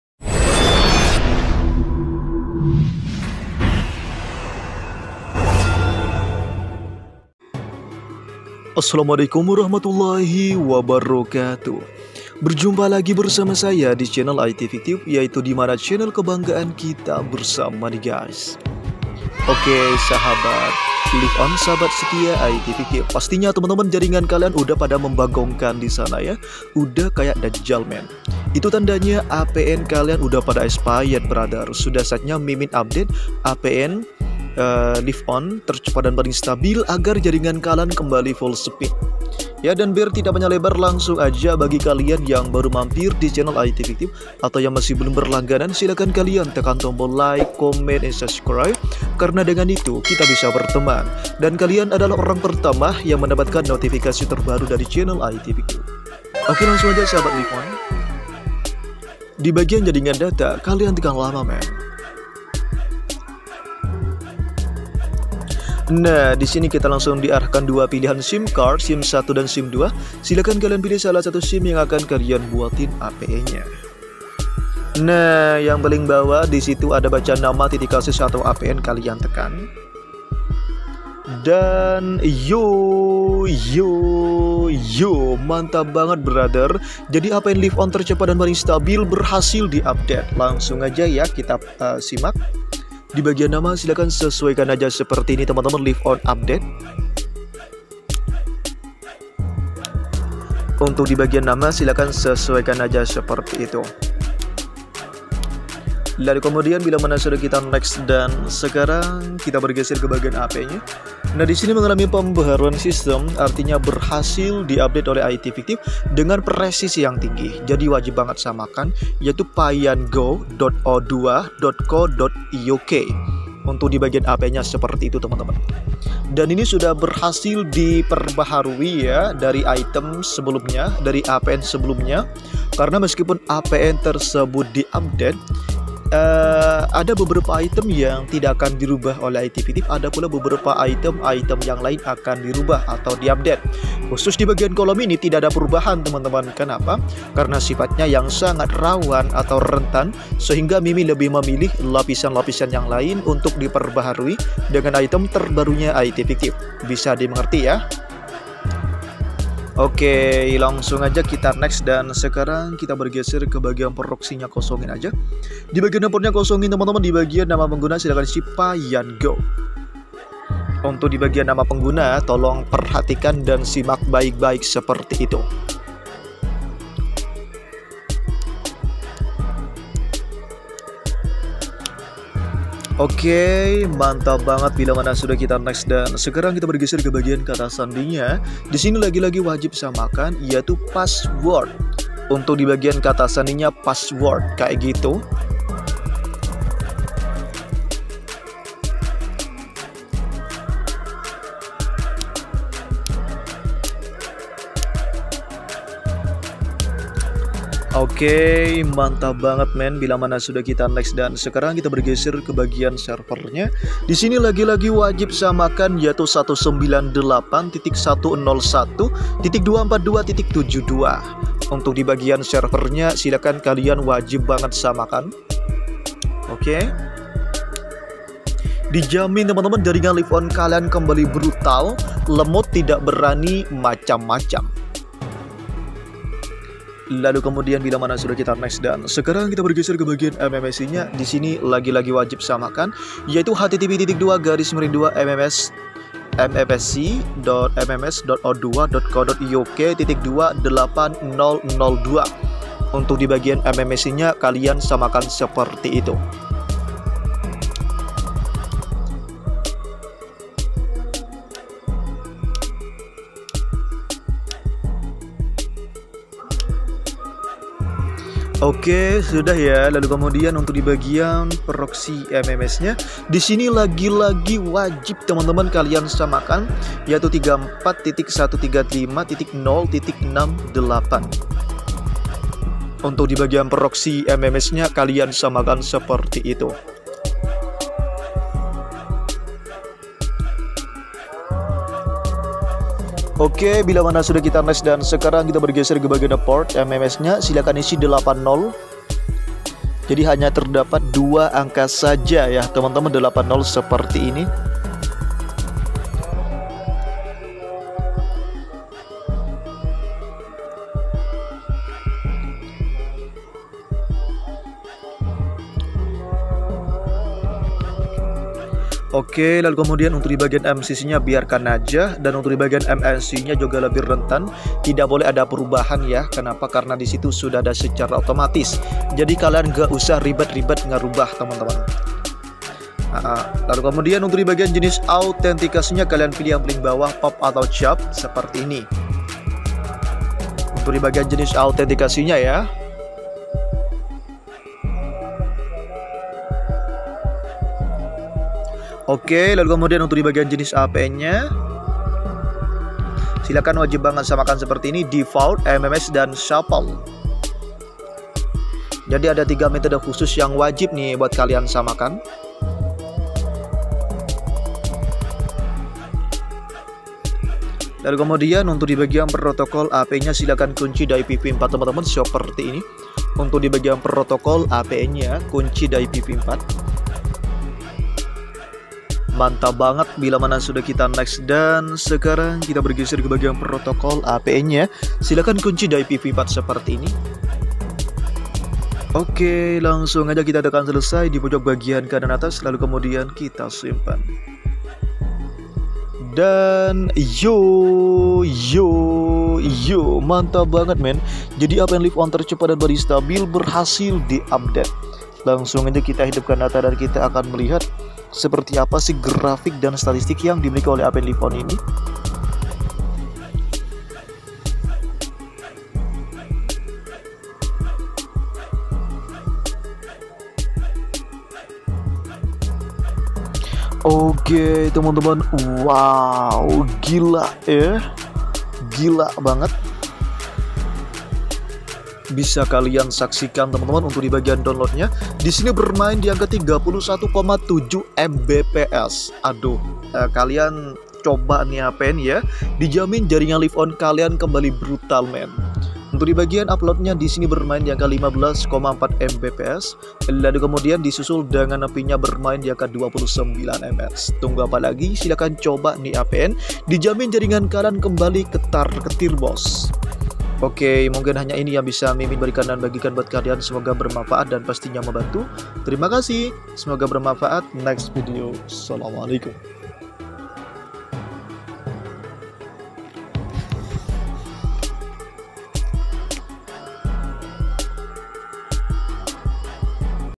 Apn Live.On Tercepat Paling Stabil Jaringan Seketika Seperti Badai Ngamuk Buruan
🔥 Vedio ini Adalah Cara Seting acces point Name Untuk Memperkuat Sinyal untuk Live.On Yang Lemot Setiap TKP Beda Rasa Jngan Takut Untuk Mencoba Semangat sahabat IT FIKTIF